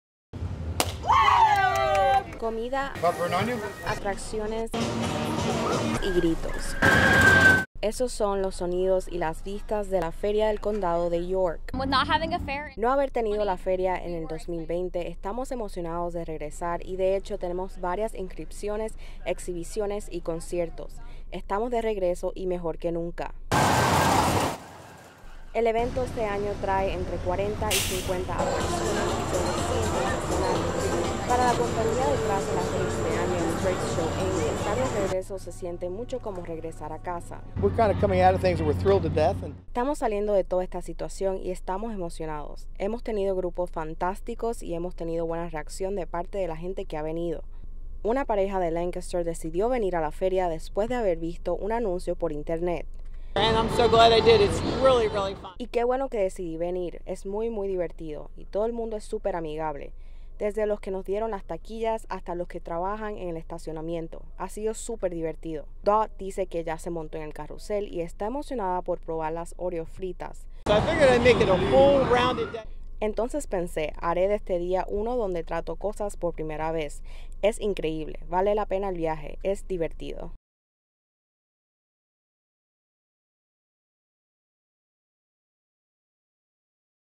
La feria del condado de York está de regreso después de dos años
Comida, atracciones y gritos. Esos son los sonidos y las vistas de la Feria del Condado de York.

“No haber tenido la Feria en 2020, estamos emocionados de regresar y de hecho tenemos varias inscripciones, exhibiciones y conciertos. Estamos de regreso y mejor que nunca,” dijo Montgomery Stambaugh, La directora de mercadeo y communicaciones de la Feria del Condado de York